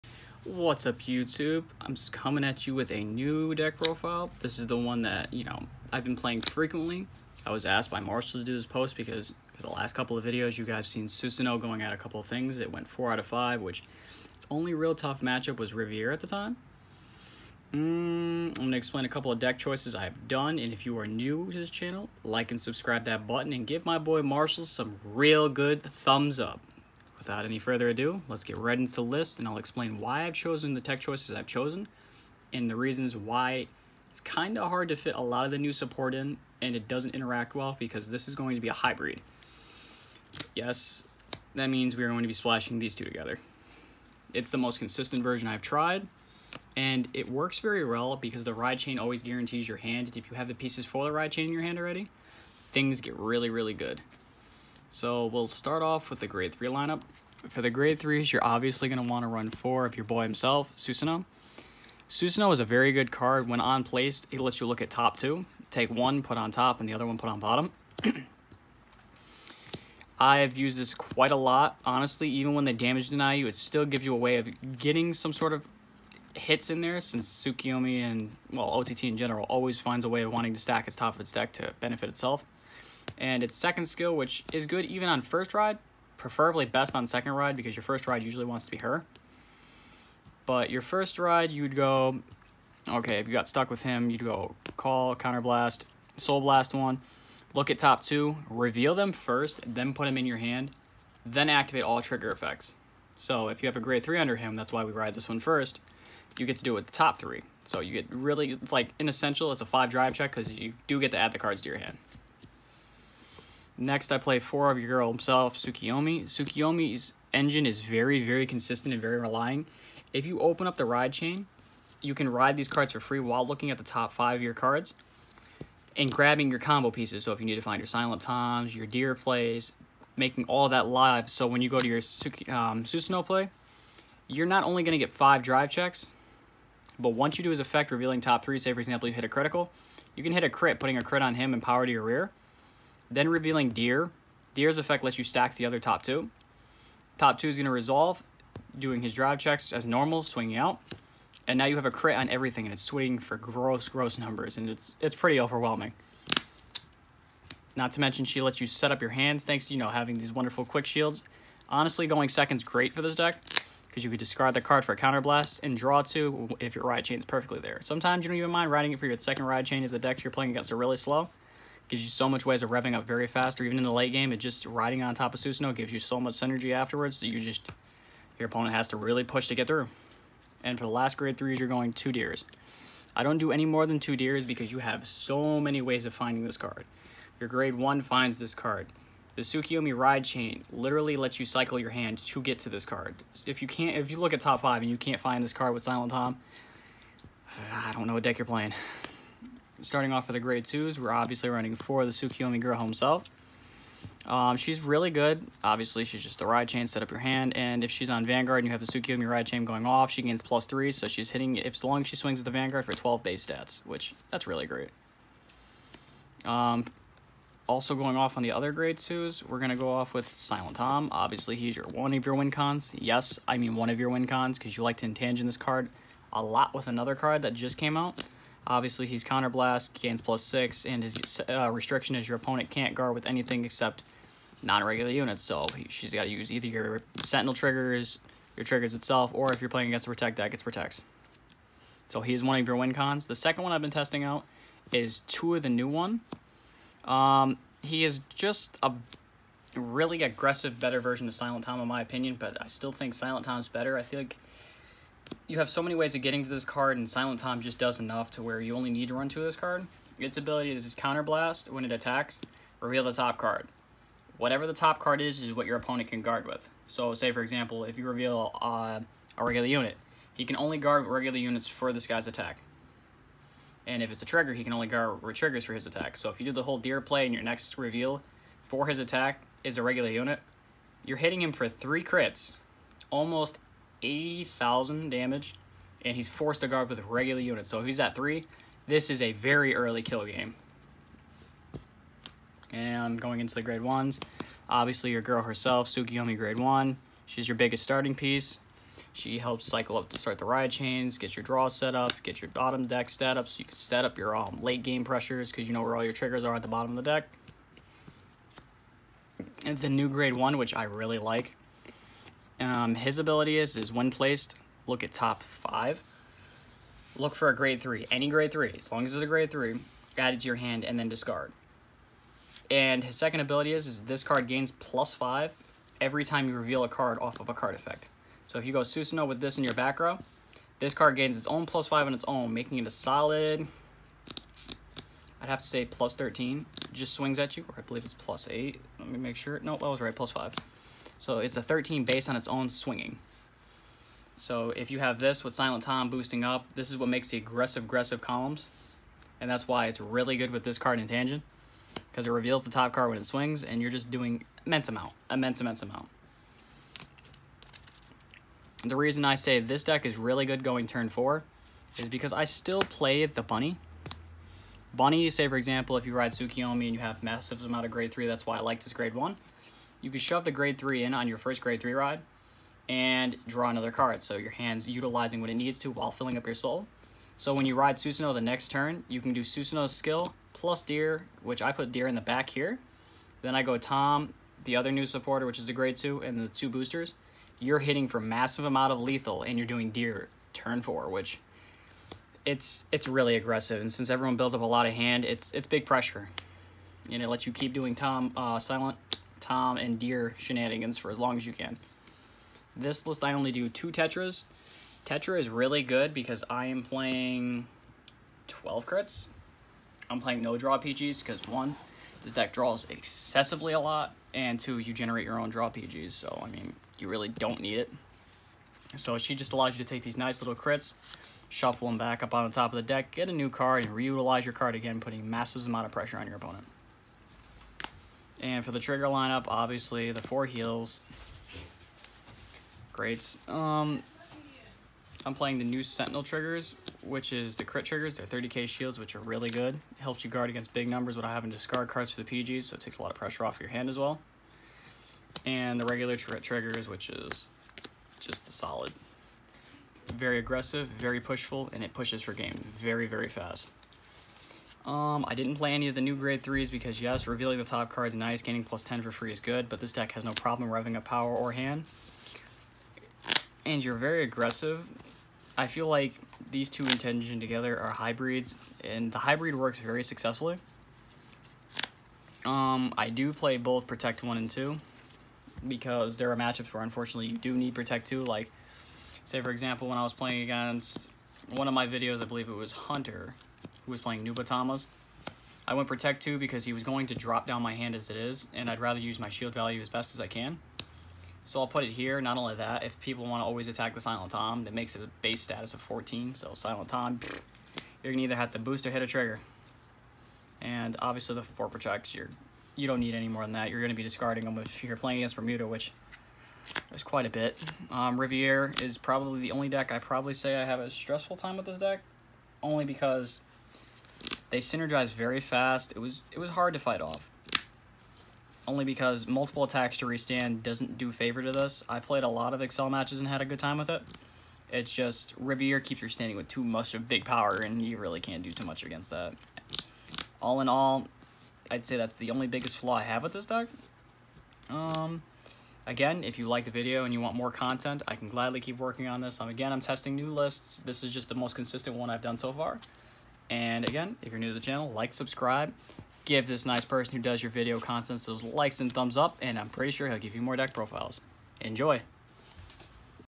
Cardfight vanguard - (Susanoo) Oracle Think Tank Deck Profile
This is the ott deck profile that has been played on the channel. It was requested to have the lists shown, so after much effort , I was able to get if for everyone to enjoy.
Also i have not forgotten about the kagero request it I'm very sorry for the delay there will probably be another deck profile video before I get to those videos due to the c word sorry and hope you enjoy